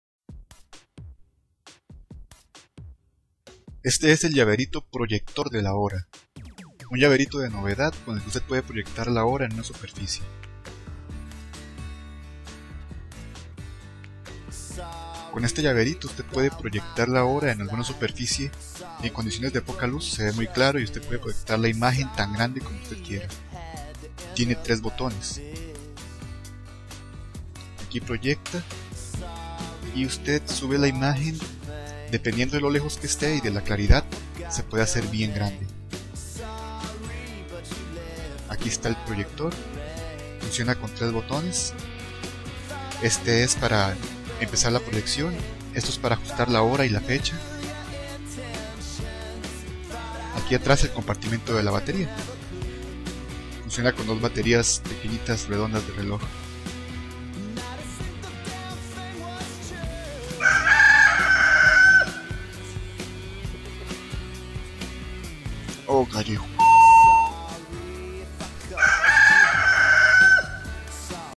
Llavero projector de la hora, novedad.
60426589
¡Este invento me cambio la vida!

Todos los que nos tenemos que levantar temprano para ir a trabajo conocemos esta situación; esta oscuro, te despiertas, no sabes que hora es, si hay levantarse o te podes quedar un rato en cama.

¿Qué haces?

1- Estiras el cuello como una tortuga para ver el reloj despertador que esta en la mesita noche y luchas para verlo si es que no lo tienes bien cerca de ti.

2- Agarras tu reloj de pulsera prendes la lucecita y luchas por discernir la hora antes de que se apague.

¡No mas!

Con este proyector de la hora solo busco la pared o el techo apunto presiono el botón y la hora aparece gloriosa(o terrible si se te hiso tarde), enorme y fácil de leer por tanto como necesite para procesarlo en mi dormida mente.

Es un regalo de novedad muy bonito para esa persona que quieres tanto y que se va a trabajar temprano.

  Para comprar 60426589.

3500 colones.